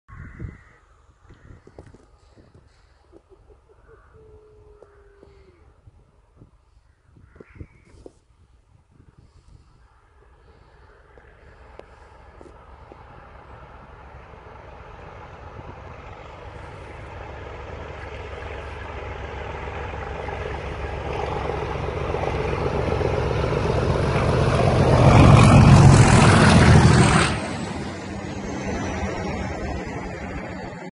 Snow plough at Strumpshaw Fen on the Norwich to Lowestoft line
Network Rail sent a snow plough to clear snow drifts from Greater Anglia's rural lines in Norfolk and Suffolk during the Beast from the East in March 2018.
Video courtesy of Network Rail.